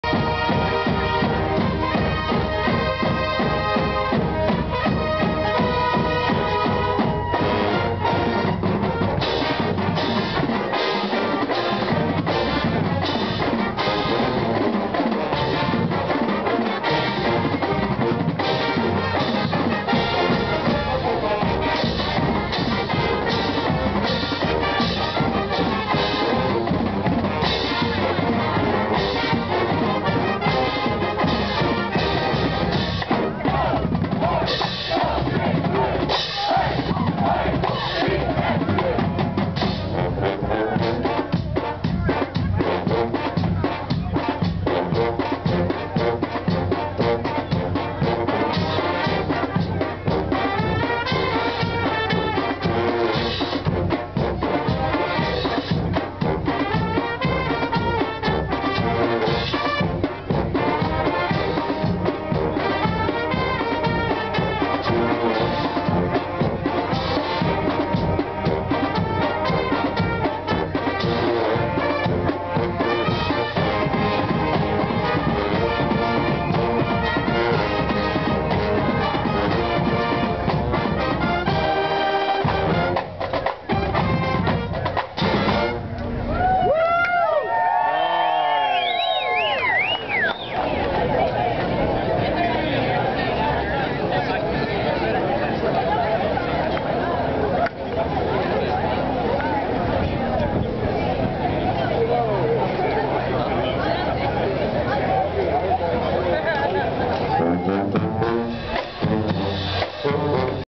boise state band playing at home depot in boise
boise state band playing at home depot in boise, beat those beavers